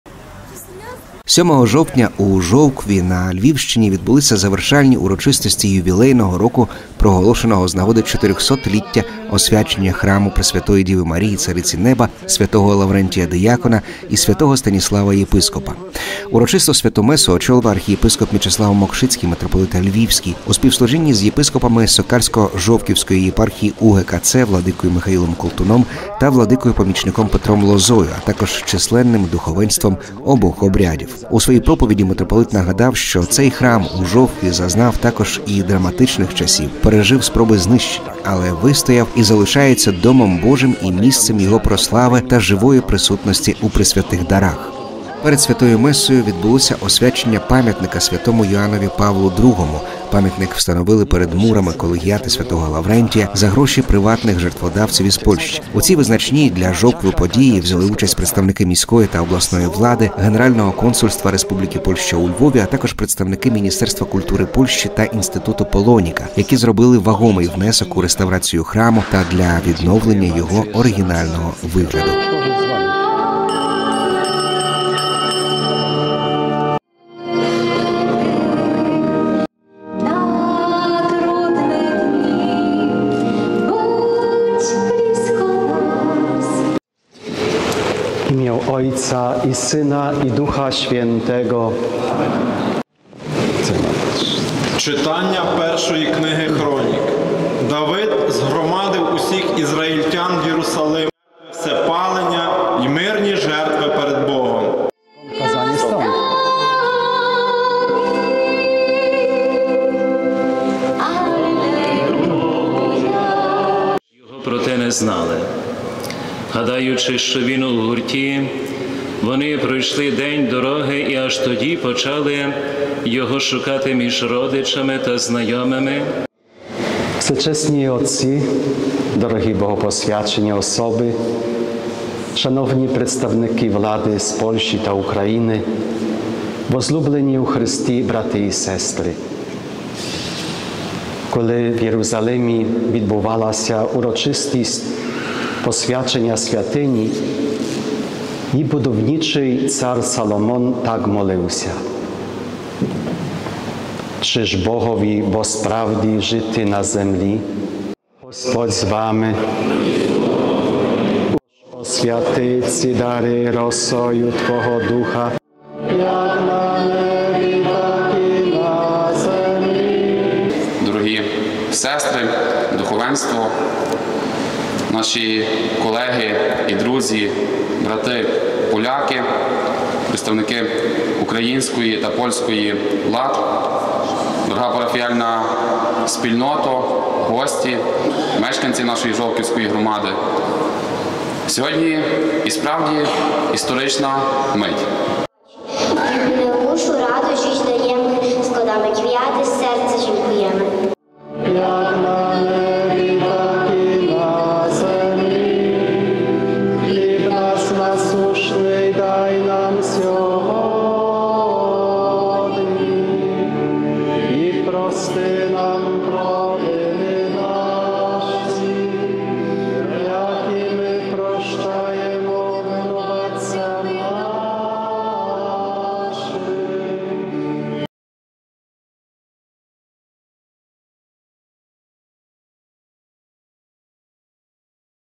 400-річчя храму в Жовкві: вчімо дітей, що це дім Божий, а не музей!
7 жовтня 2023 р., у Жовкві на Львівщині відбулися завершальні урочистості Ювілейного року, проголошеного з нагоди 400‑ліття освячення храму Пресвятої Діви Марії Цариці Неба та св. Лаверентія Диякона і св. Станіслава Єпископа.

Урочисту Святу Месу очолив архієпископ Мечислав Мокшицький, митрополит Львівський, у спіслужінні з єпископами Сокальсько-Жовківської єпархії УГКЦ владикою Михаїлом Колтуном та владикою-помічником Петром Лозою, а також численним духовенством обох обрядів.

У проповіді митрополит нагадав, що цей храм зазнав також і драматичних часів, пережив спроби знищення, — але вистояв і залишається домом Божим, місцем Його прослави і живої присутності у Пресвятих Дарах:

— Після чотирьохсот років існування та надзвичайної історії цього храму, давайте скажемо собі та майбутнім поколінням, що ми повинні поклонятися Богові в цьому місці, тому що Господь Ісус живе тут у дарохранительниці, прихований у Пресвятих Дарах; що Він удень і вночі чекає на нас і на наше поклоніння. Тож, входячи до храму, не сприймаймо його лише як музейний об’єкт, але, вірячи в живу присутність Бога, перехрестімося свяченою водою, щоб нагадати собі, що це дім Божий, а тоді схилімо коліна, щоби вклонитися Господеві зі словами: «Нехай буде прославлена Пресвята Господня Тайна, істинне Тіло і Кров Господа нашого Ісуса Христа!» Навчаймо цього дітей, щоб вони знали, що це Божий дім і що тут треба Богу поклонятися. Нагадуймо про це молоді і ніколи не забуваймо про це, щоби своєю недбалістю не осквернити святу присутність Бога.

Ми маємо приходити сюди, щоб дякувати Богові за все. Щиро дякувати за цей храм, за те, що Бог живе серед нас. За наших чудових предків, які довірилися Богу, і за приклад їхньої живої віри.(…) Маємо також огорнути своєю вдячністю і молитвою тих, хто допомагає нам відновити цей храм, повернути йому його красу і велич.

Святій Месі передувало освячення пам’ятника св. Йоанові Павлу ІІ. Пам’ятник встановили перед мурами Колегіати св. Лаврентія за кошти приватних жертводавців із Польщі. У цій визначній для Жовкви події взяли участь представники міської та обласної влади, Генерального консульства РП у Львові, а також представники Міністерства культури Польщі та Інституту Polonika, які зробили вагомий внесок у реставрацію храму та відновлення його оригінального вигляду.

Храм Цариці Неба, св. Лаврентія диякона і свмуч. Станіслава єпископа в Жовкові постав у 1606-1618 роках. Фундатором будівництва був гетьман Станіслав Жолкевський. За його задумом, Колегіата св. Лаврентія мала відображати всю велич лицарської слави епохи Відродження. За часів правнука гетьмана — короля Яна ІІІ з роду Собеських, храм став родинним мавзолеєм і місцем багатьох важливих державних церемоній. Жовківська Колегіата досі зберігає численні пам’ятки лицарського минулого і є свого роду пантеоном польської воєнної слави. До 1939 року вона вважалася одним із трьох найважливіших храмів Речі Посполитої.

1946 року, з приходом совєтів, храм перетворили на склад мінеральних добрив. Відтоді він почав зазнавати дедалі більших руйнувань та ушкоджень. Через знищені дерев’яні конструкції даху почали замокати склепіння, через що елементи ліпного оздоблення відпадали. Радянська влада знищила орган, зазнали сильних пошкоджень статуї, барельєфи надгробків; було розбито більшість пам’ятних таблиць, пограбовано поховання у крипті, знищено надгробки. Ікону XVII ст. авторства художника Карло Дольчі «Богородиця з Немовлям» із головного вівтаря у 1953 р. вивезли до Ермітажу в Ленінграді.

Храм повернули вірянам лише 1989 року. І від наступного, 1990-го над реставрацією Колегіати працюють польські фахівці. Роботи здійснюються завдяки коштам польського Фонду охорони пам’яток. На сьогодні вже відновлено значну частину давнього оздоблення храму. Деякі елементи інтер’єру, такі як картини із зображенням батальних сцен, хоч і вдалося врятувати, але вони ще не повернулися до храму. Перші два з чотирьох славнозвісних батальних полотен були написані на замовлення самого гетьмана Жолкевського: це «Битва під Клушином» та «Битва під Хотином», а два інші — «Битва під Віднем» та «Битва під Парканами» — замовив король Ян ІІІ Собеський. Сьогодні ці полотна належать Львівській картинній галереї. На жаль, війна завадила перемовинам щодо їхнього повернення на стіни храму Жовківської Колегіати.